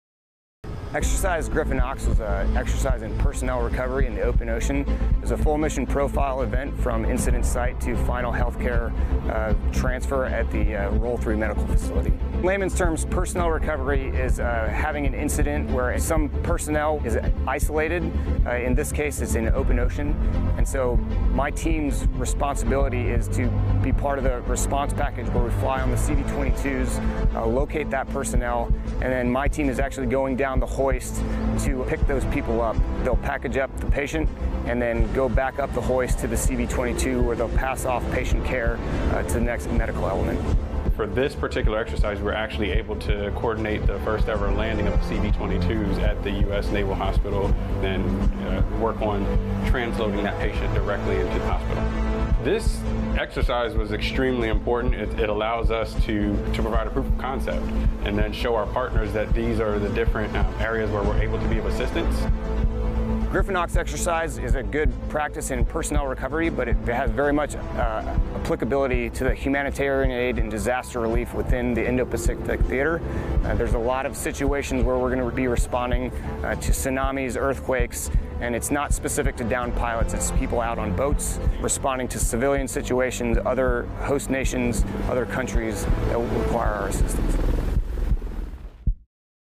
U.S. Naval Hospital Okinawa (USNHO) participate in Exercise Gryphon Ox 21-1, March 24, 2021.
Commando Medics
CAMP FOSTER, OKINAWA, JAPAN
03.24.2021

U.S. service members from 320th Special Tactics Squadron (STS), 353rd Special Operations Support Squadron (SOSS), 21st Special Operations Squadron (SOS), and the U.S. Naval Hospital Okinawa (USNHO) participate in Exercise Gryphon Ox 21-1, March 24, 2021, on Okinawa, Japan. During the exercise, para-rescue personnel from the 320th STS and medical personnel from the 353rd SOSS were transported by the 21st SOS via CV-22 to recover and treat a simulated plane crash casualty in the open ocean at night before transferring the casualty to care at the USNHO. This exercise included the first-ever landing of a CV-22 at the USNHO and provided a proof of concept for a real-world contingency plan, showing our partners in the region our capabilities for support in case of natural disasters or emergencies. (U.S. Marine Corps video by Cpl. Carla O)

Film Credits: Video by Cpl. Carla O
AFN Okinawa

Special Operations
AFSOC
Commandos
Medics
353rd SOG
USHNO